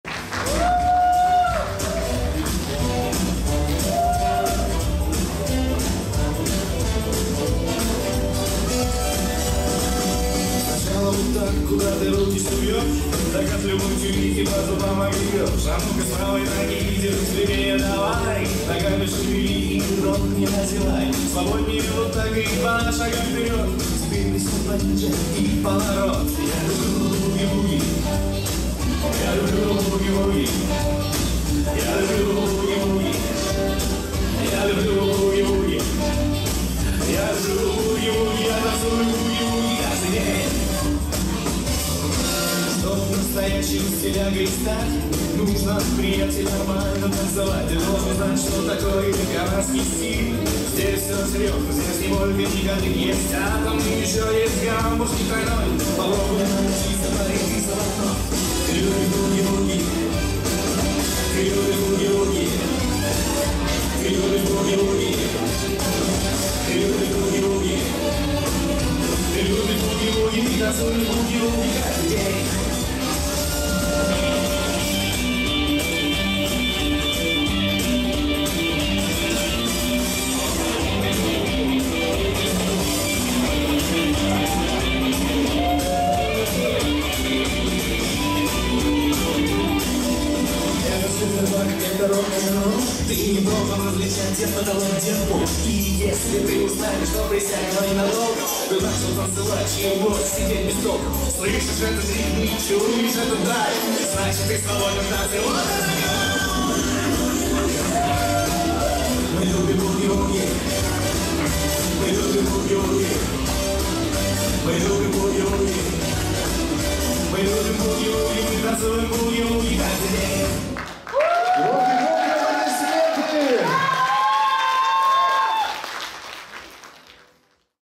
Творческий номер - Дарьи Корпачевой
Творческий номер - Дарьи Корпачевой, участницы под номером 5, конкурс «Мисс Краса 2019»
В «Затишье» прошел финал детского конкурса красоты и талантов «Мисс Краса 2019», подробнее